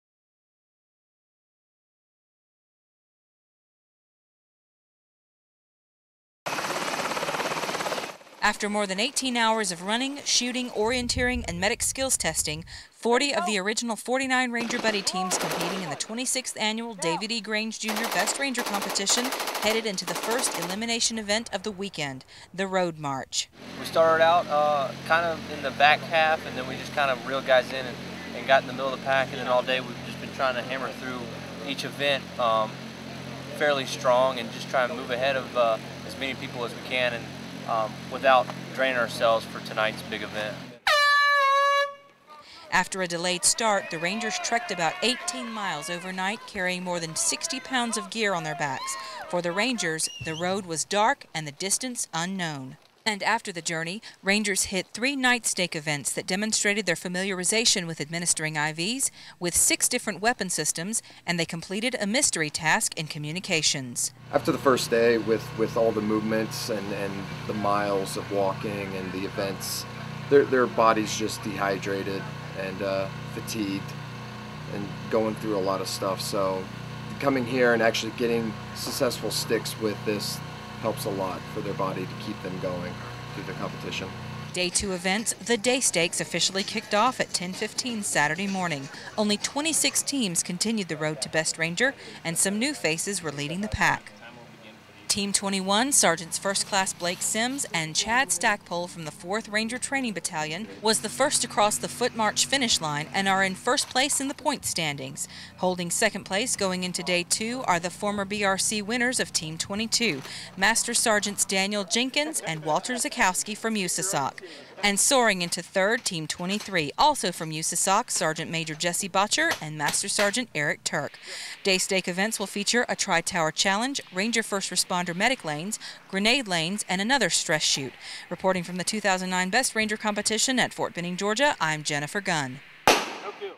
Best Ranger Competition 2009 Day 2
Footage of the 2nd day of the 2009 Best Ranger Competition at Fort Benning, GA.